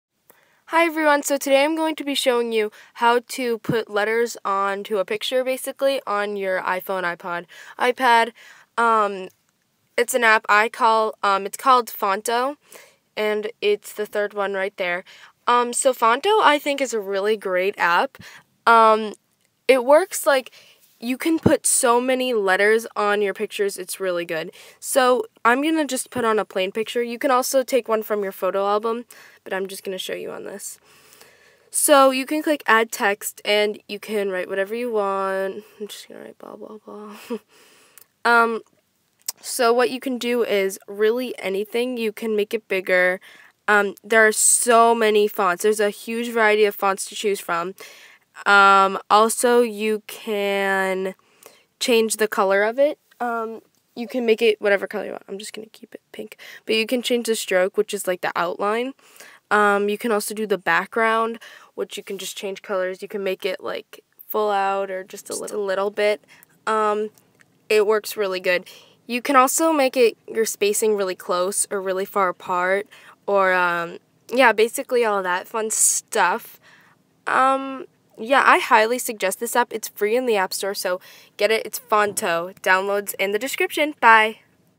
Phonto
Do want a text to picture application? This one is for you! With a huge variety of fonts and colors and sizes, you'll want more!